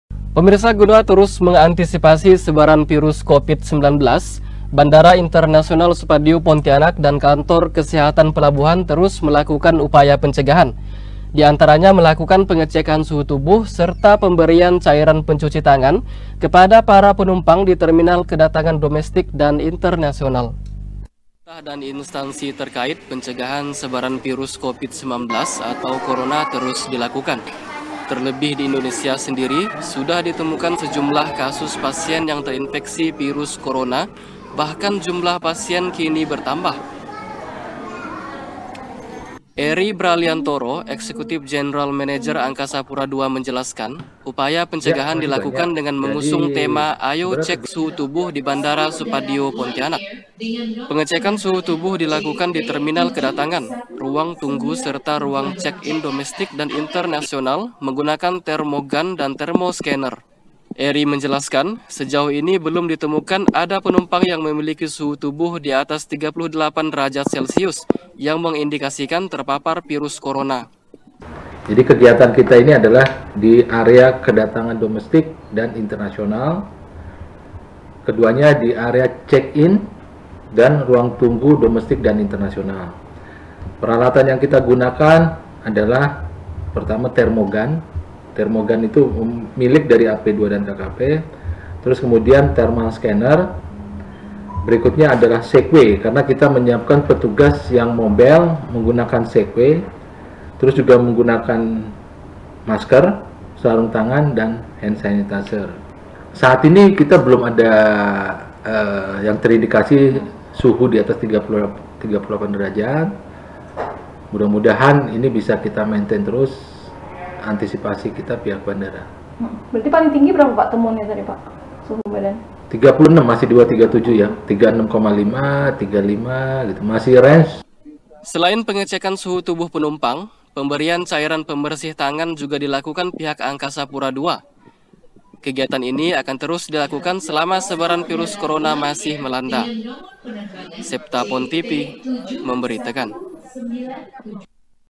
Cek Suhu Tubuh di Terminal Kedatangan Waspada Corona
Guna terus mengantisipasi sebaran virus Covid-19, Bandara Internasional Supadio Pontianak dan Kantor Kesehatan Pelabuhan terus melakukan upaya pencegahan. Di antaranya melakukan pengecekan suhu tubuh serta pemberian cairan pencuci tangan kepada para penumpang di terminal kedatangan domestik dan Internasional